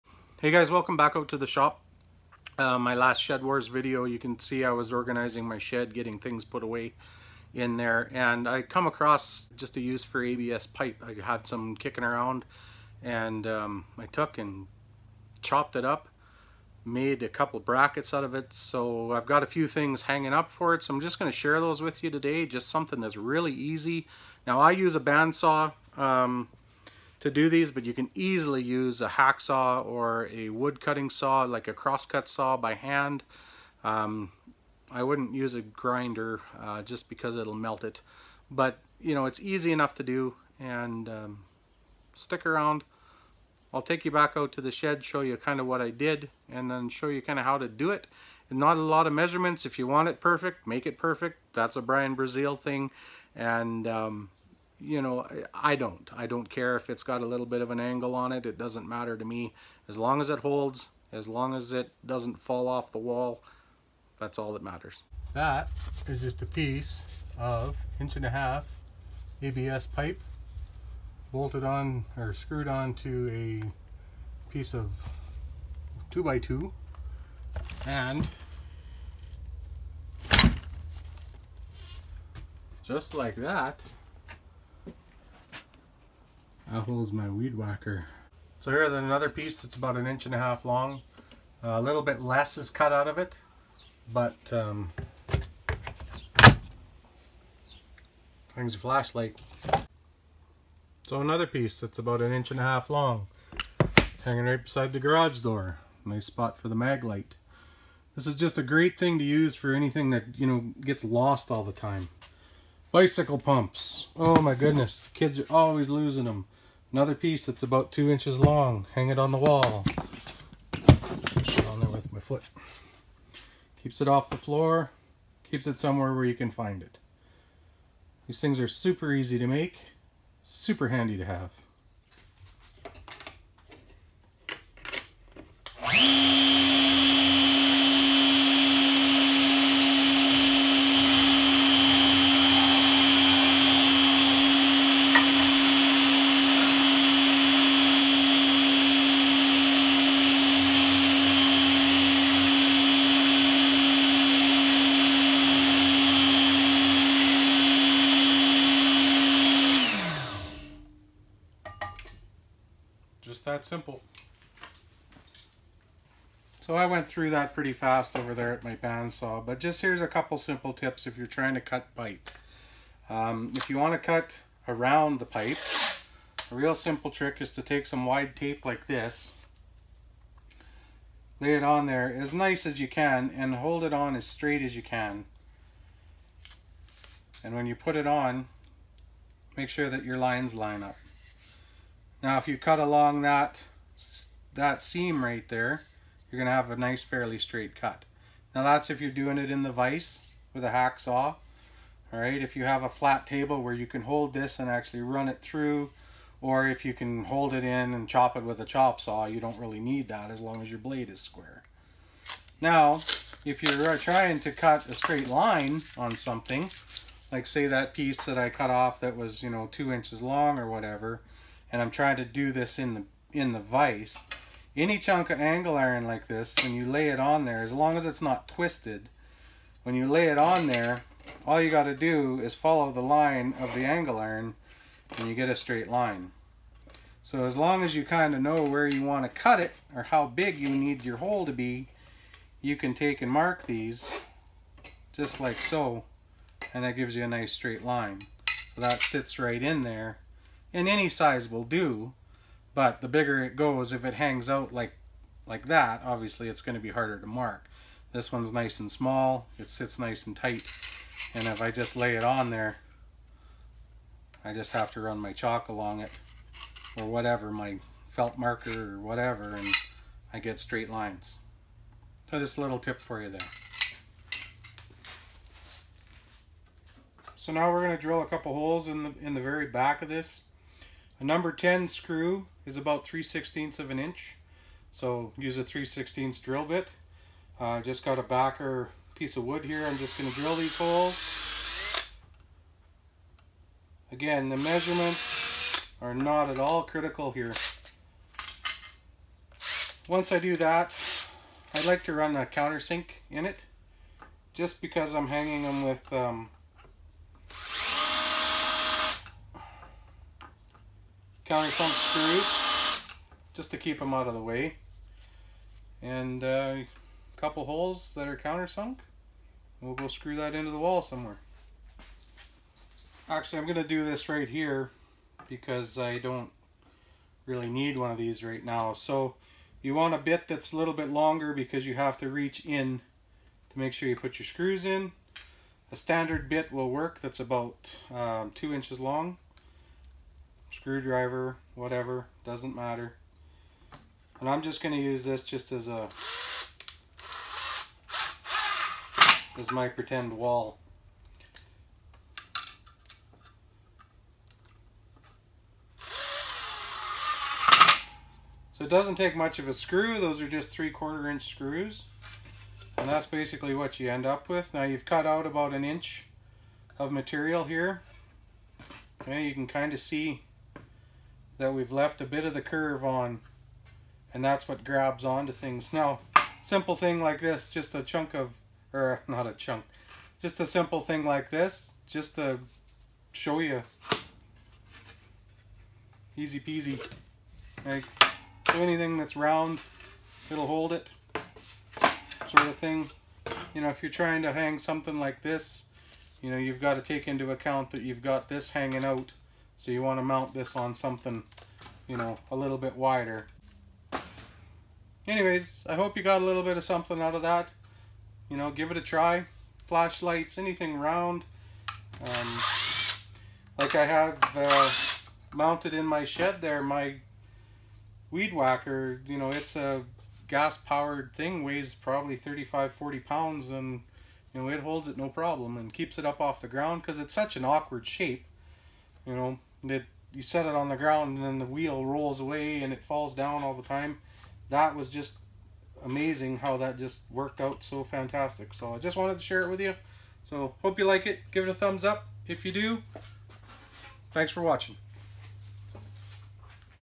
Getting Organized with Cheap Brackets // ABS Pipe
Simple brackets made from scrap ABS pipe to hold things for easy access or hang awkward things that are hard to store. Brackets made for pennies. Save money and get organized.